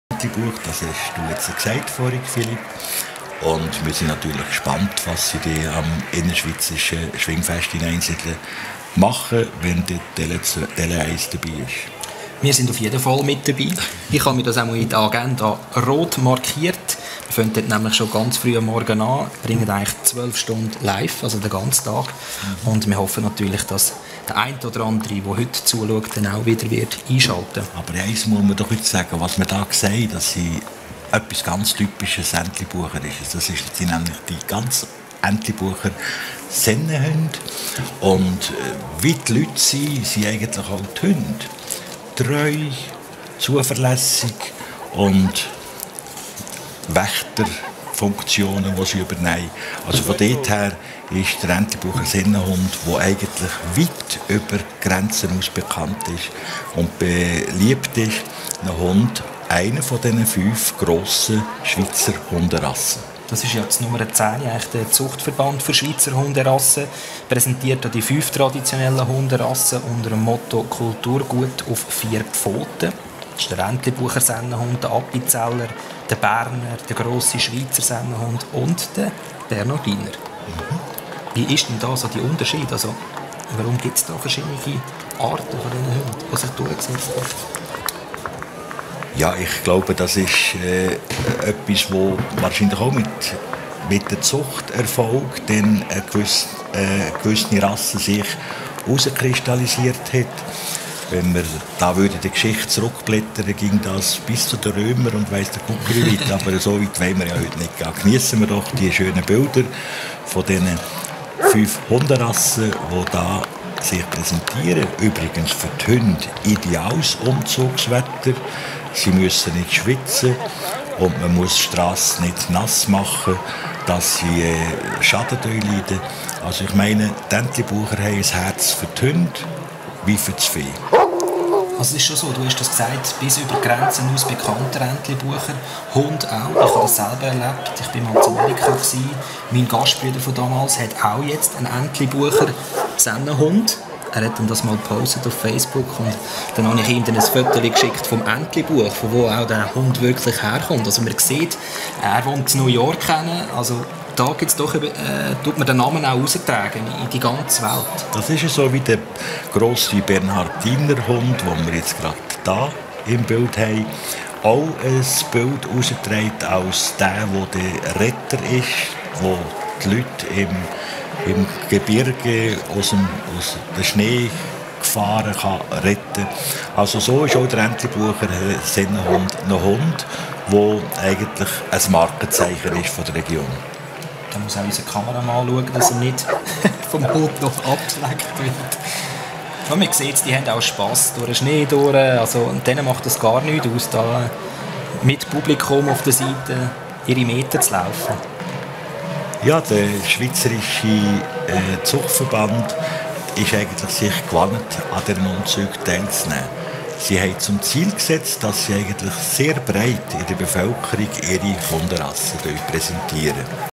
Jodlerfestumzug Schüpfheim
«Kulturgut auf vier Pfoten», unter diesem Motto präsentieren sich Entlebucher, Appenzeller, Berner und Grosse Schweizer Sennenhunde sowie die Bernhardiner jeweils an den schweizerischen Brauchtumsumzügen. Am 26. Juni 2016 fand in Schüpfheim, im Herzen des Entlebuchs, der Umzug des Zentralschweizer Jodlerfests statt - ein Heimspiel für unsere Entlebucher!